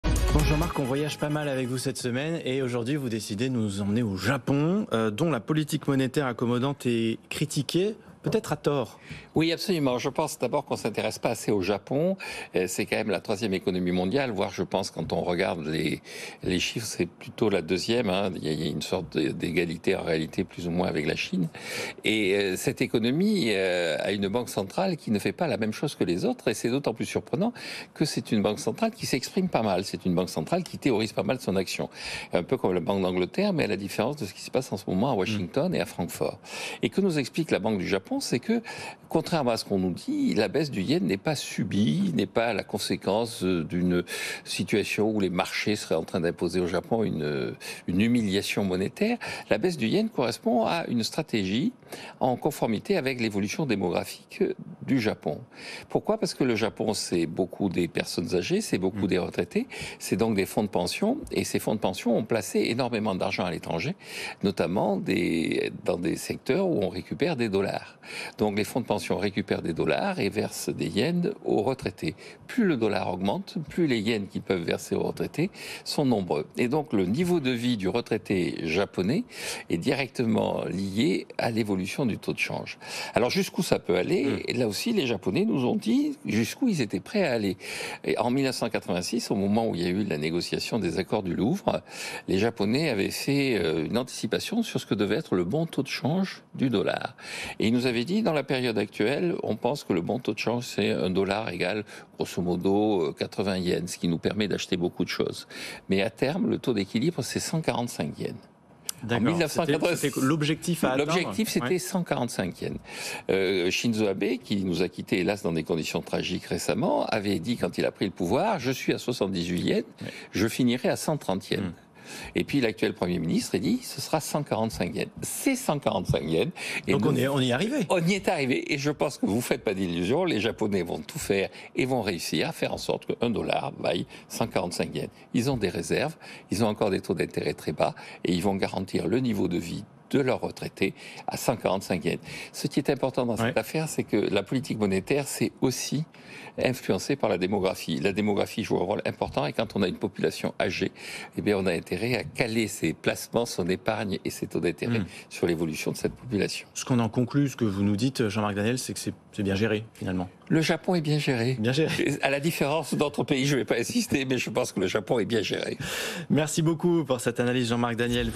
Japon: la politique monétaire accommodante est critiquée
Jean-Marc Daniel: “La baisse du yen correspond à une stratégie en conformité avec l’évolution démographique du Japon”